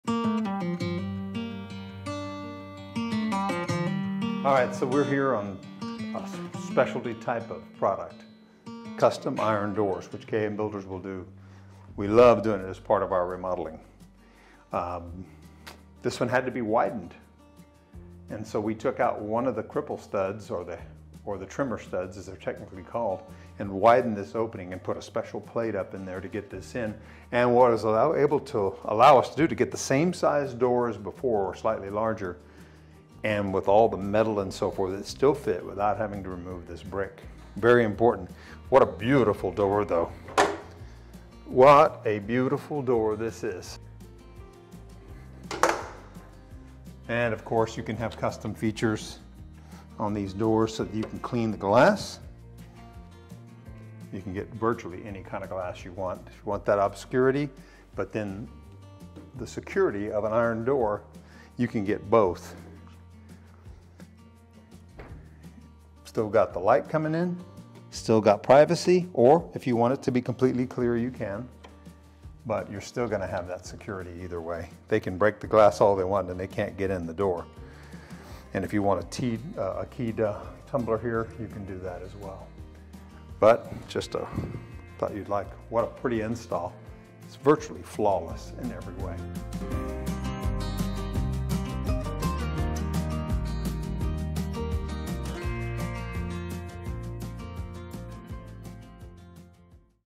KM DETAILS - Iron Door Details Ep. 34
In today's episode of KM DETAILS, Keith shares the beauty of these incredible iron doors. Our team was able to help our clients create this gorgeous custom design that brought our client's vision to life.